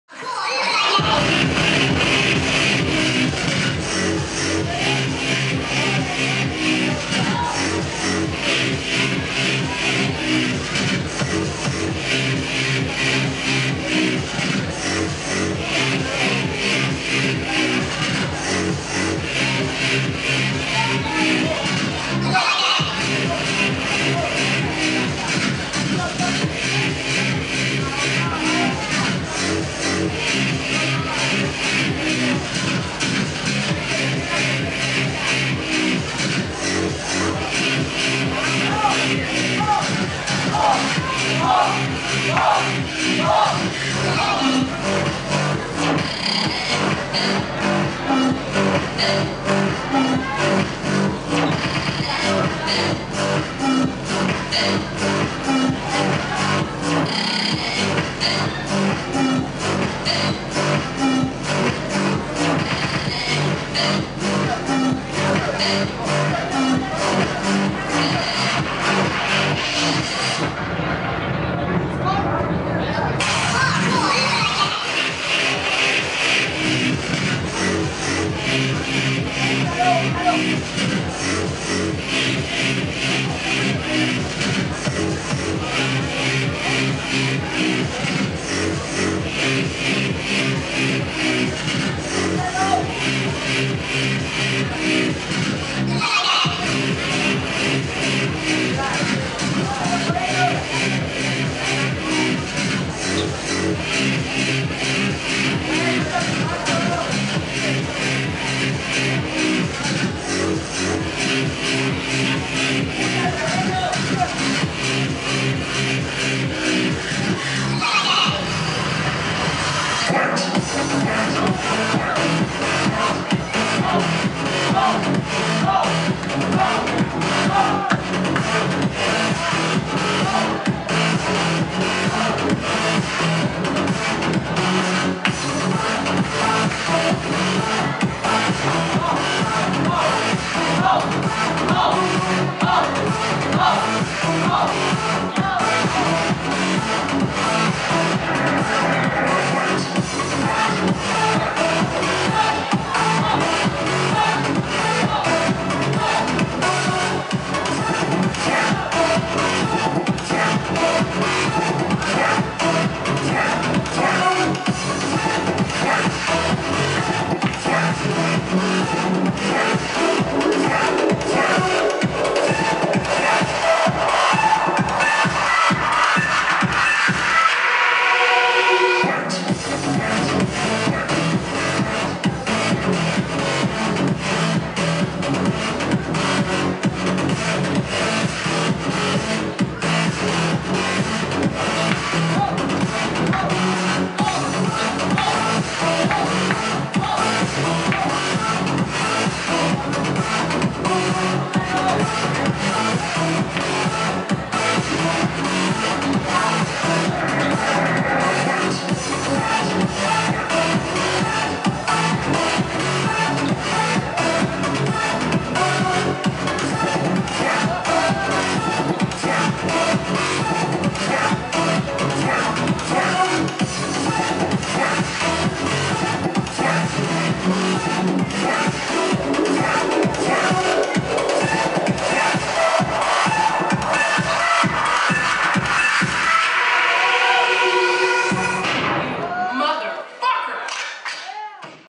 Omar (DV) vs Moreno (DIP) | Semifinal Cat. Advance | Soul 2, Cancún, Q. Roo
Ganador: Moreno, Dance Is Passion.